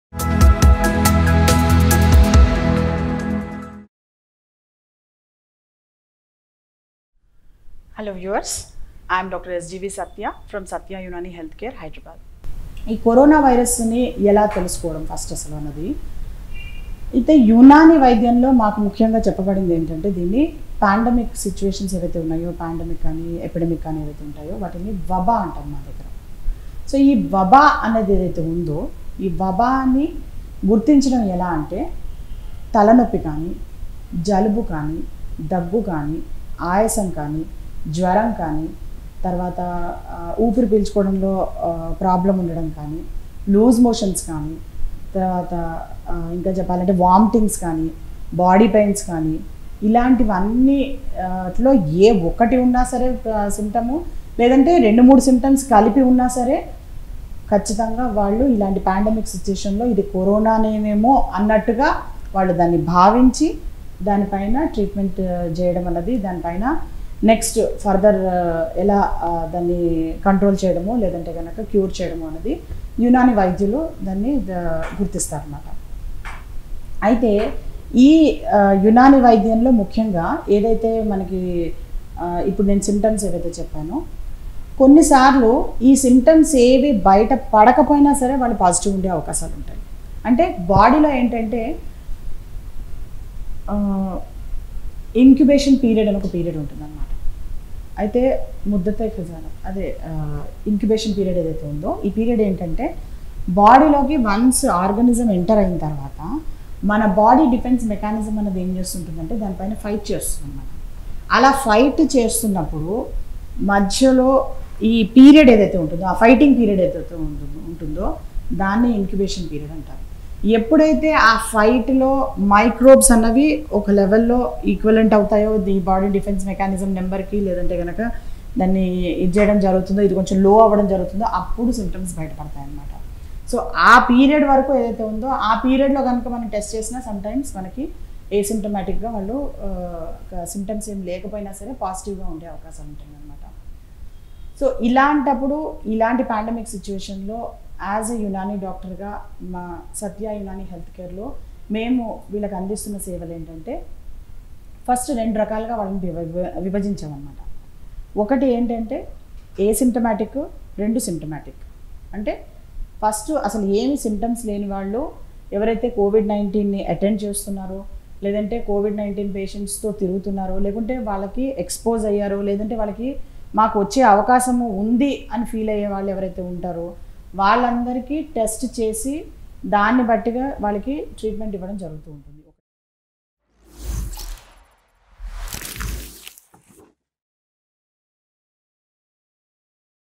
How to Identify the Symptoms of Coronavirus in Telugu || Na Arogyam
In This Video Dr SGV Sathya Explains us How to Identify the Symptoms of Coronavirus in Telugu,By Unani Medicine